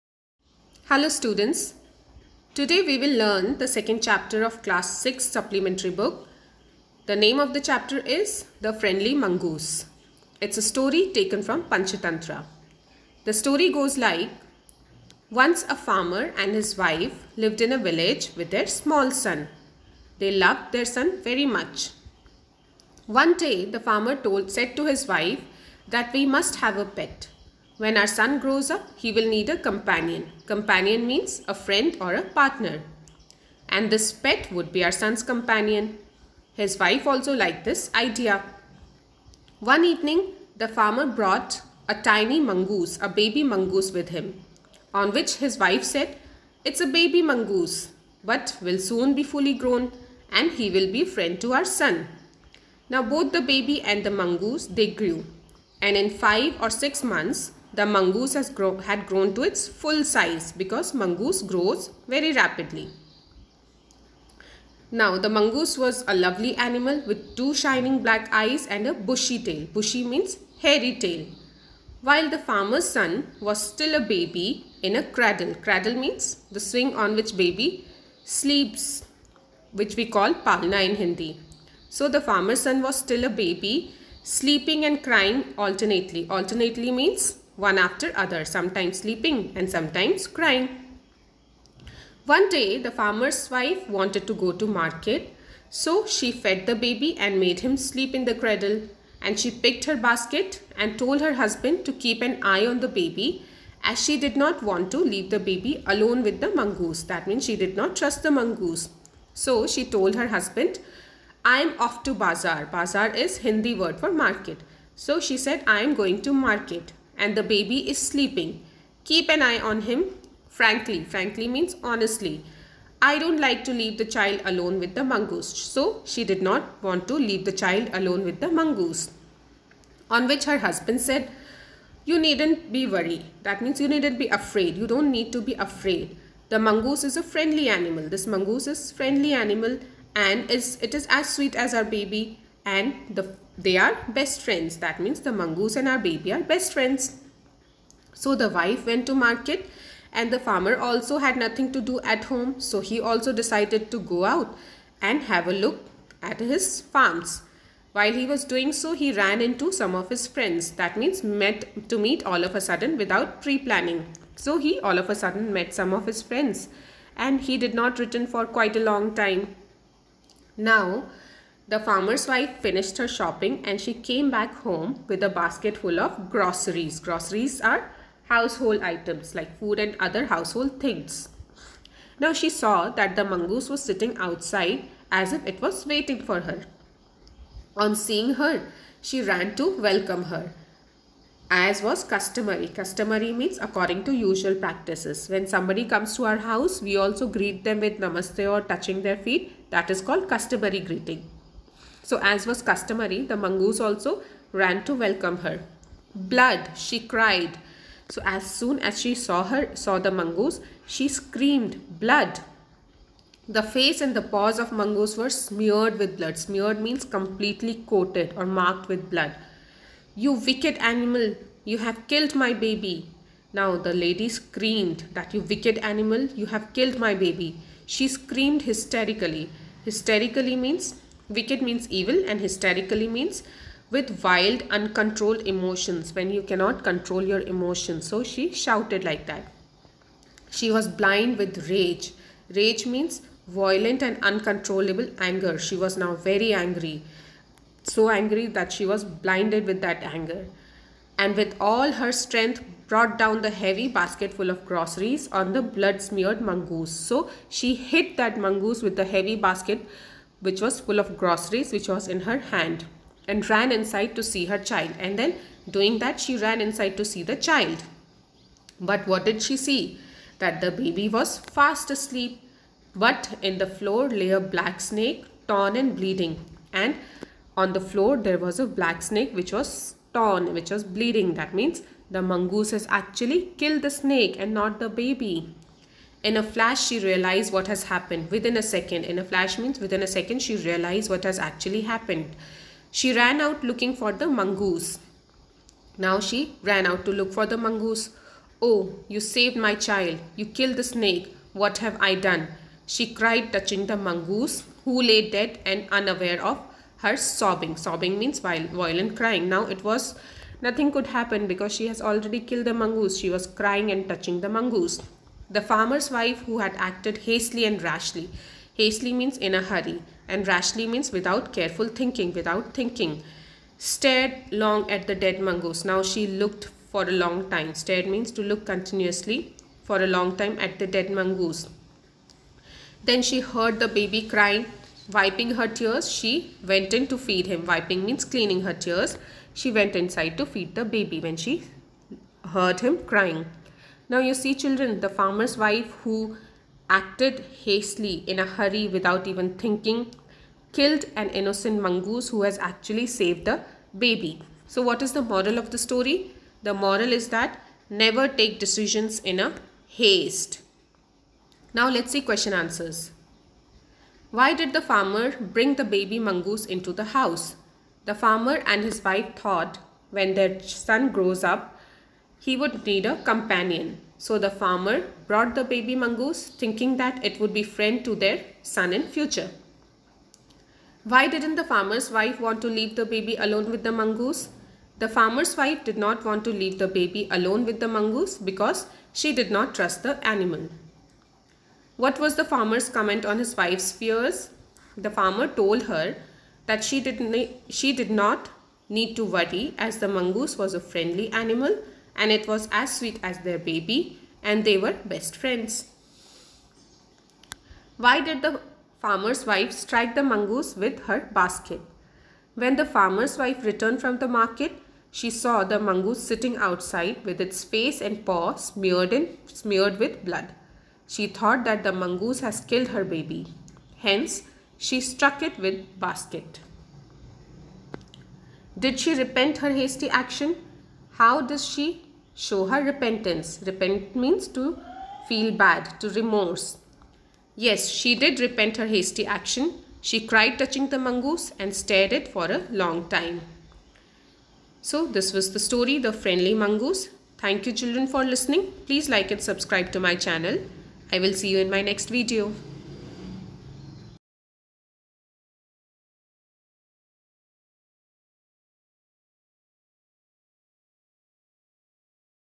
The Friendly Mongoose Class 6 in English , A pact with the Sun Chapter 2
The video is on the second chapter of Class 6 Supplementary  book "The Pact With The Sun" the story is "The friendly Mongoose"  which is taken from The Panchatantra. the video gives complete English explanation, vocabulary and question answers on the chapter